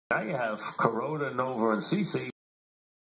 Mike Francesa Mentions Novar
Da Mighty Francesar reminds his listnizz and viewizz dat da Yankees have Kuroda, Novar, and CC.

Long live Da New Yawk Spawtz Pope.